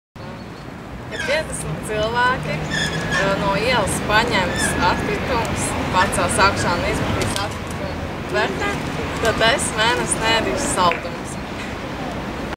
Ja Tu pacelsi atkritumus un iemetīsi tam paredzētajā vietā, Agnese mēnesi neēdīs saldumus!
Ja vismaz 50 cilvēki kaut reizi pacels kādus uz ielas esošus atkritumus un iemetīs tos atkritumu tvertnē, Agnese centīsies mēnesi iztikt bez saldumiem! Pieņem izaicinājumu, un Zemes stundā 31. martā 20:30 - 21:30 izslēdz gaismu!